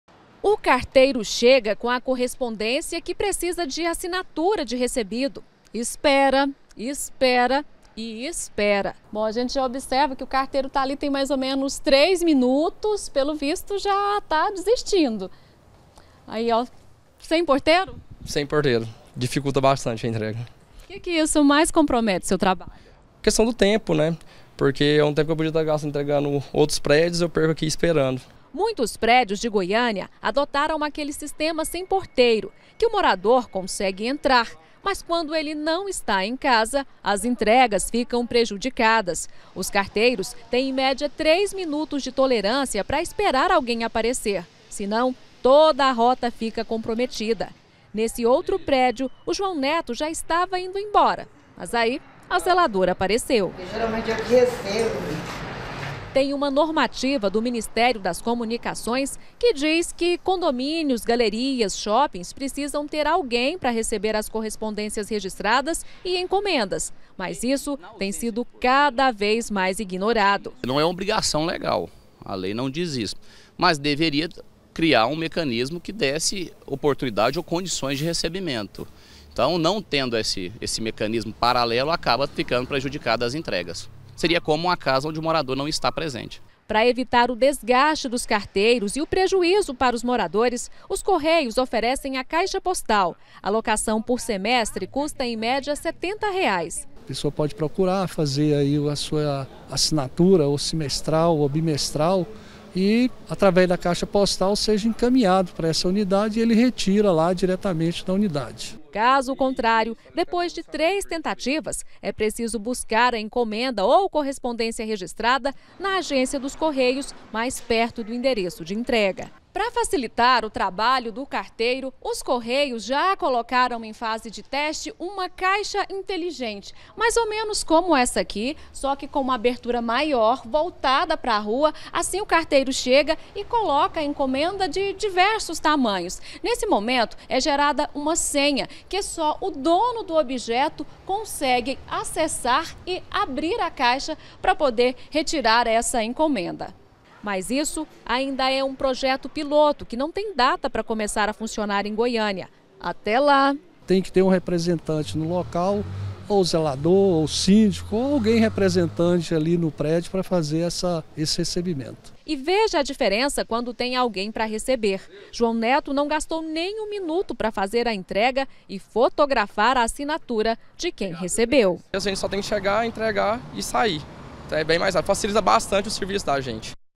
CORRESPONDÊNCIAS: PRÉDIO SEM PORTEIRO DIFICULTA ENTREGA
A gente começa mostrando um problema cada vez mais comum em Goiânia. Carteiros têm dificuldade pra entregar correspondências em prédios sem porteiros. De um tempo pra cá, vários condomínios adotaram o sistema de vigilância eletrônica, sem funcionário. Quando o carteiro chega, não tem como entregar a encomenda.